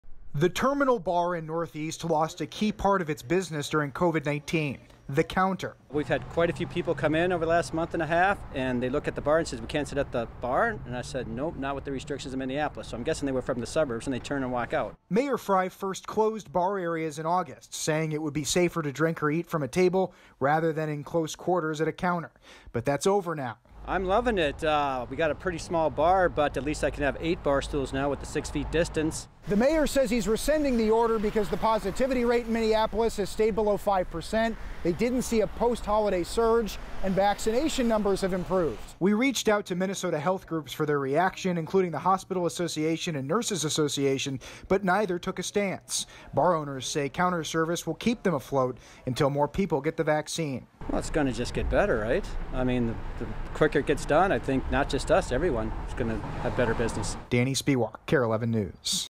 Mayor Jacob Frey allows bar areas to reopen in Minneapolis
The mayor says COVID-19 metrics have improved, making the restrictions no longer necessary.